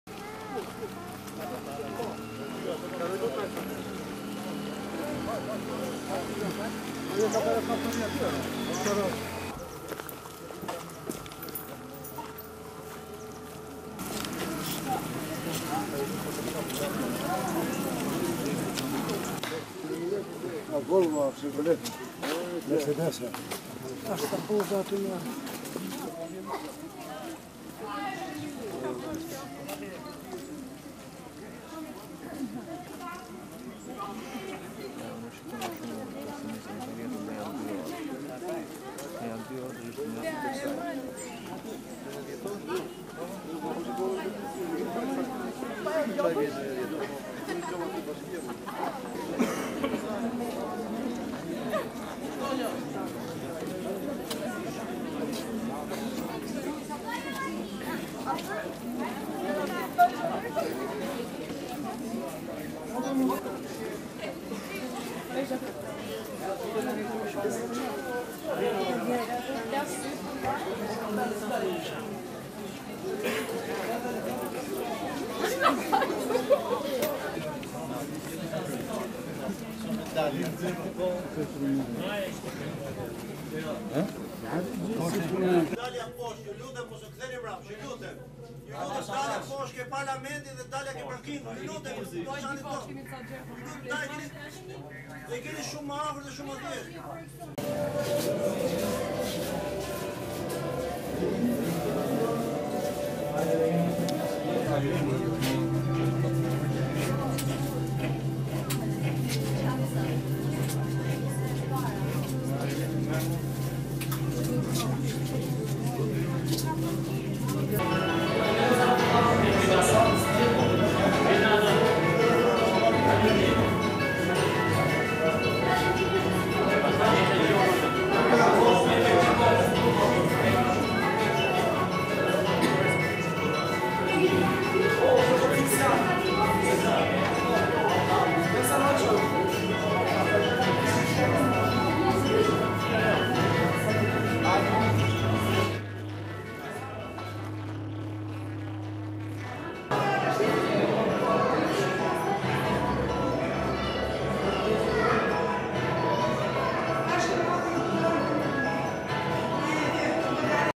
A1 Report - Bunk’Art thyen pritshmëritë, njerëzit radhë për të parë dhomën e Enverit
Në një fundjavë me shi, mijëra qytetarë kanë zgjedhur të kalojnë disa orë pushimi duke parë ambientet e Bunk’Art, bunkerit antibërthmor i ndërtuar në regjimin komunist.

Nostalgjikë të këtij regjimi, por edhe të tjerë të shtyrë nga kureshtja për të parë se çfarë është një bunker kanë bërë që Bunk’Art të mirëpres mijëra vizitorë në këtë ditë të dielë.

Dhoma e projektuar për të qëndruar ish-diktatori Enver Hoxha dhe ajo e kryeministrit Mehmet Shehu, janë dy nga dhomat e tërheqin më shumë kureshtjen e qytetarëve.

Në dhomën e Enver Hoxhës degjohen pjesë nga fjalimet, në zyrën e punës është gazeta Zëri i Popullit dhe më pas krevati në dhomën e fjetjes.

Ekspozimet vijojnë në dhoma ku projektorët tregojnë për luftën, po ashtu edhe kur janë të ekspozuar veshjet ushtarake.

Të duhen minuta të tëra që mes labirinthesh e dyersh të betonta të arrish drejt daljes së Bunk’Art.